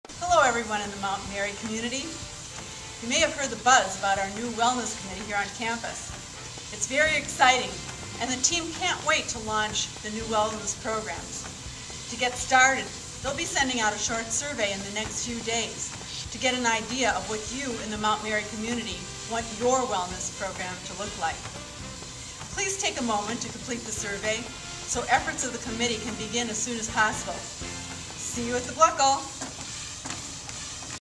Mount Mary College Wellness Survey
Mount Mary College President Eileen Schwalbach invites the Mount  Mary community to participate in a wellness program survey.